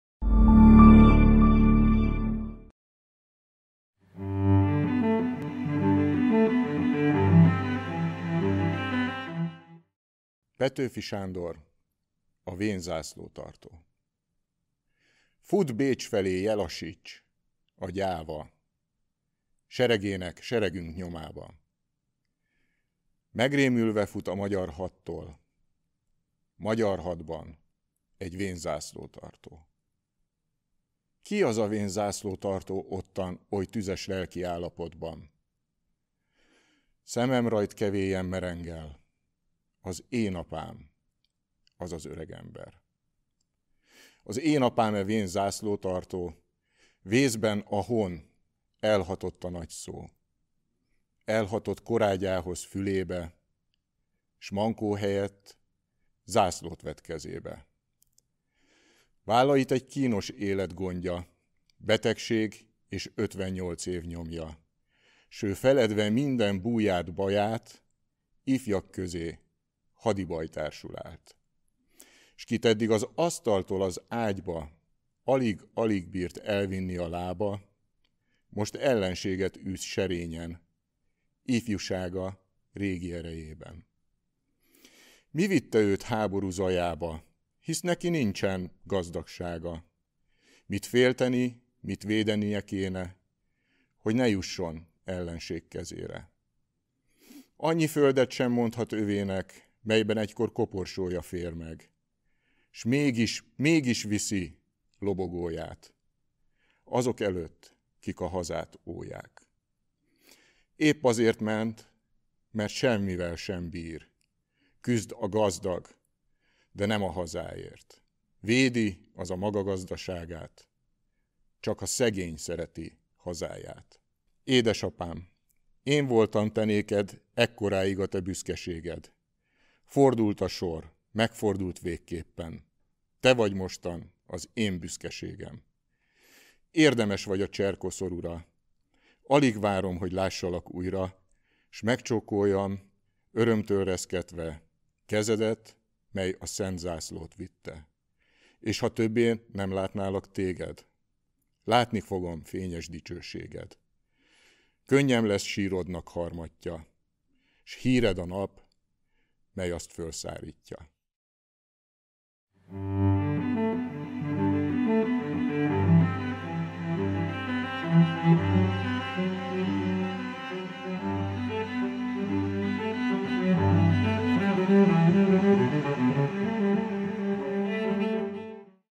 Kedvenc versem: Aranyos László | Petőfi Sándor: A vén zászlótartó
Online verses programsorozatunk következő részében Aranyos László szavalását hallhatjuk.

Várjuk azokat a versszerető és versmondó barátainkat, akik szívesen elmondják kedvenc versüket és vállalják, hogy a videofelvételt megosszuk.

2021